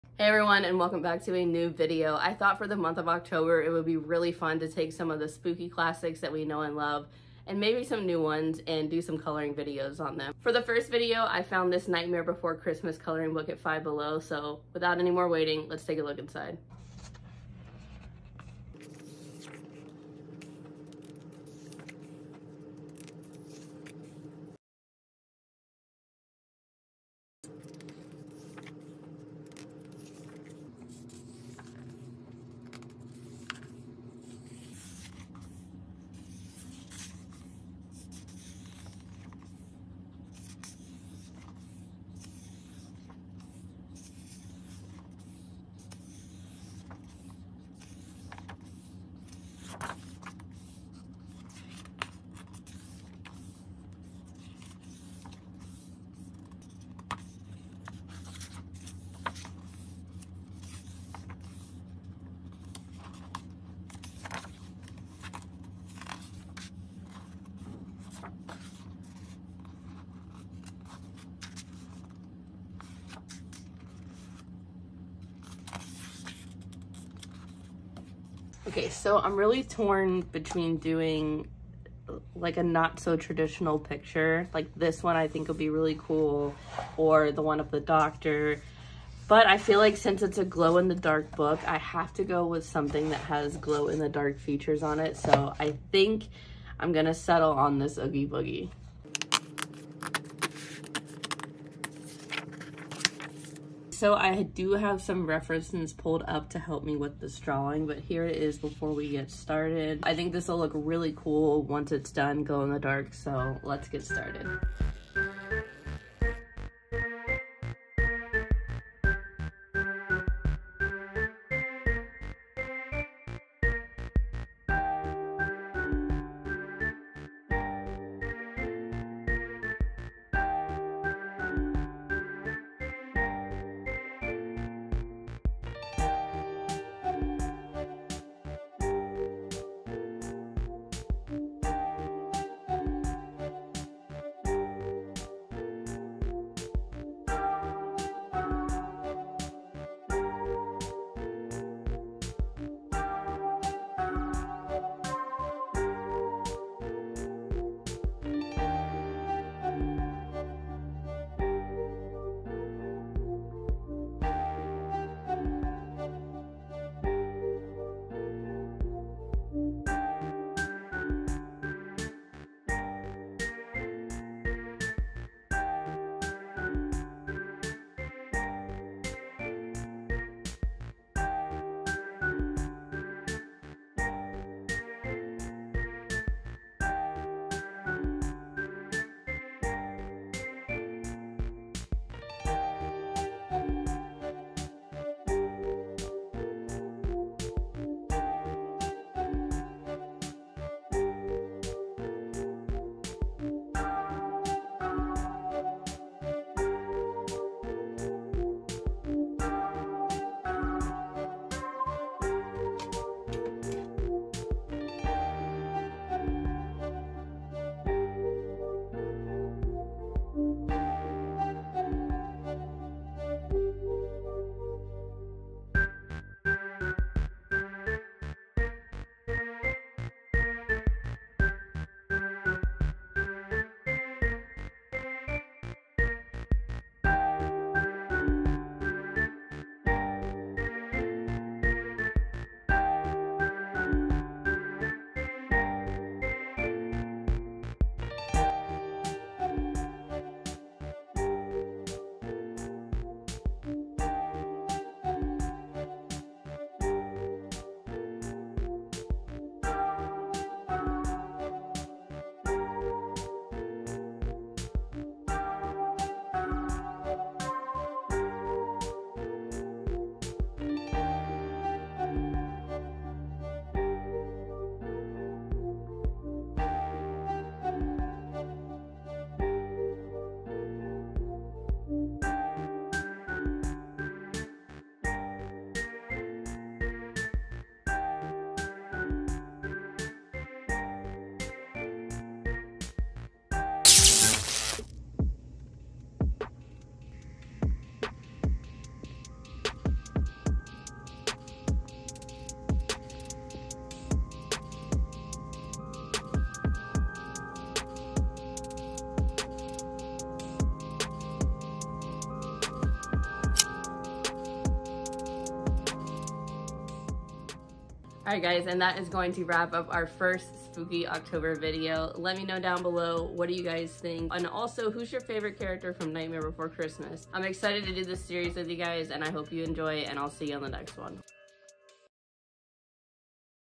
THE NIGHTMARE BEFORE CHRISTMAS coloring of Oogie Boogie‼️ (Halloween series PART 1)
Coloring in The Nightmare Before Christmas glow-in-the-dark book!

Coloring Book:

Markers:
Ohuhu Alcohol Brush Markers -...

Colored Pencils:
Prismacolor Premier Colored...